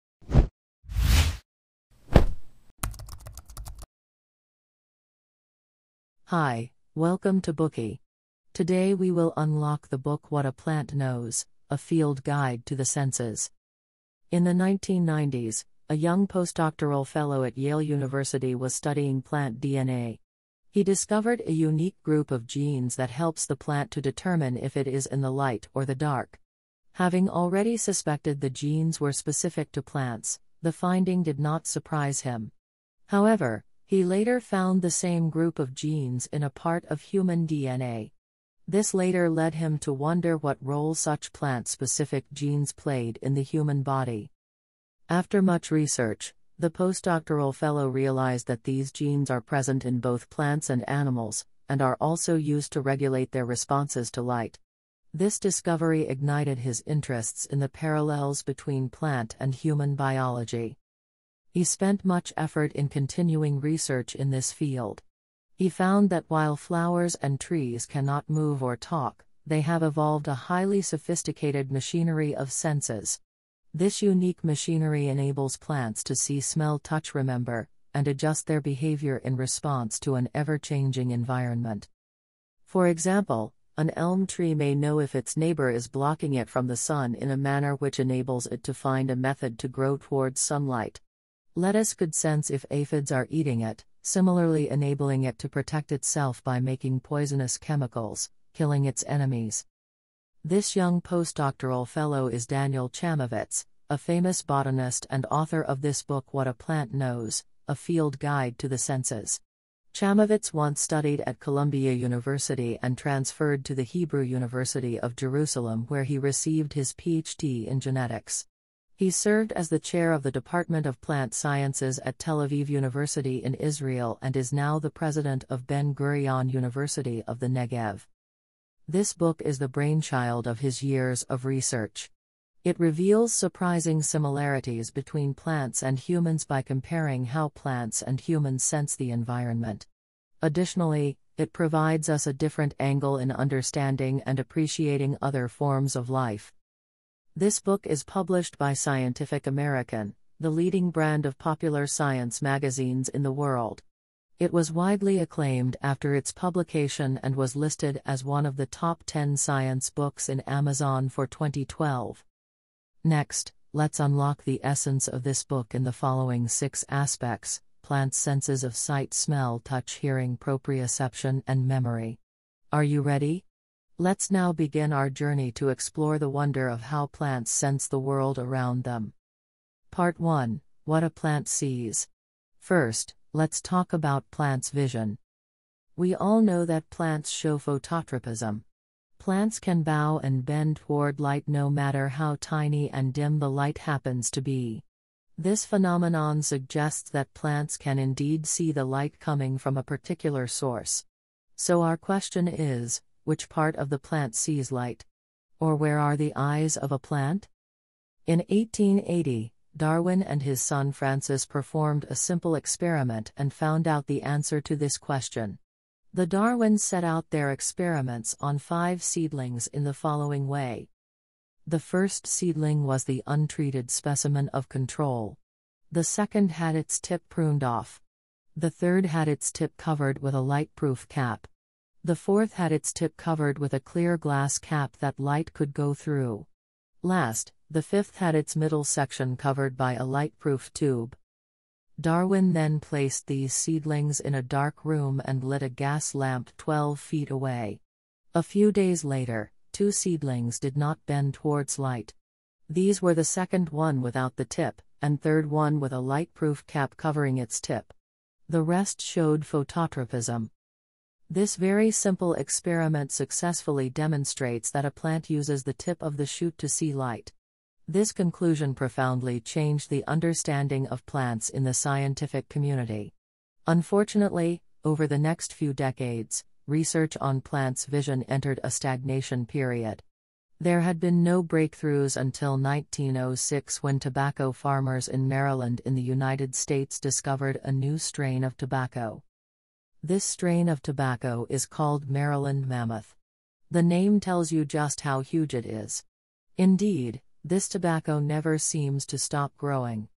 What a Plant Knows Book Summary By Daniel Chamovitz A Field Guide to the Senses
What a Plant Knows: A Field Guide to the Senses is a science book published by the Scientific American, the world’s leading popular science magazine. It has been widely praised after its publication, and was listed as Amazon’s Top 10 Science Books in 2012. This book uncovers the mystery of plants. It examines whether plants have the senses of sight, smell, touch, hearing, proprioception, and even memory, and helps us comprehensively understand plants’ inner world.

Daniel Chamovitz is a world-class botanist. He once studied at Columbia University and later transferred to The Hebrew University of Jerusalem, where he received his doctorate in genetics. He served as the Chair of the Department of Plant Sciences at Tel Aviv University in Israel, and is now the President of Ben-Gurion  University of the Negev. His work on plants and fruit flies has been published in top scientific journals, and this book is the product of many years of work in this field.